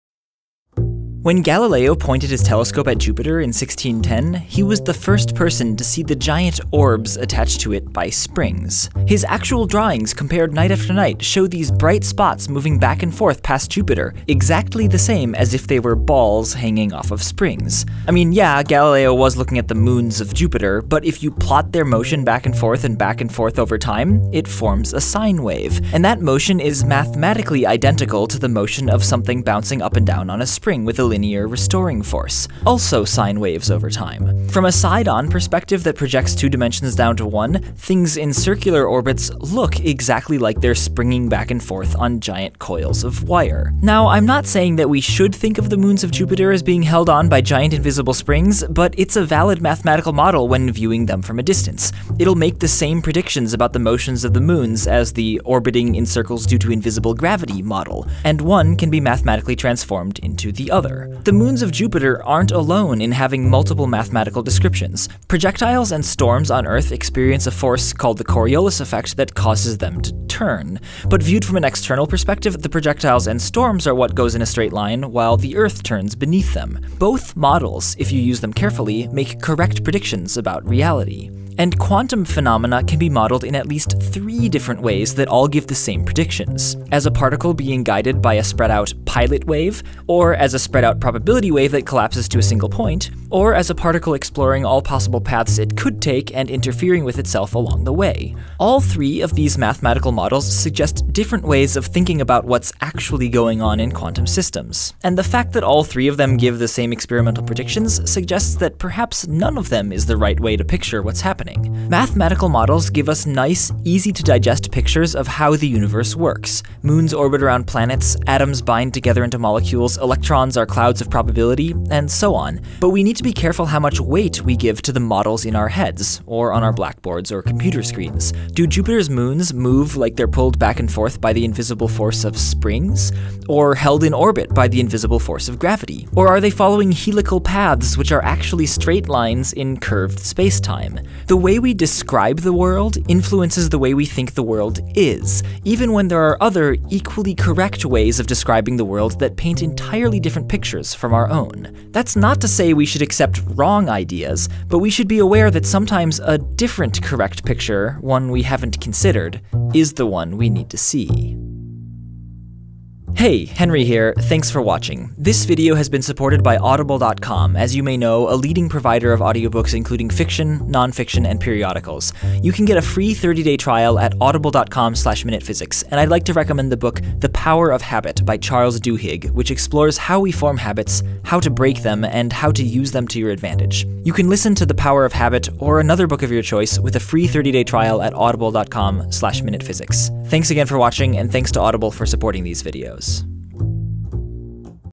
How Perspective Shapes Reality
for supporting this video, which is about how the way we describe the world can influence the way we perceive it. In particular, with regards to Bohmian mechanics, Schrodinger wave functions, Feynman path integrals, and Galilean moons attached to Jupiter by springs.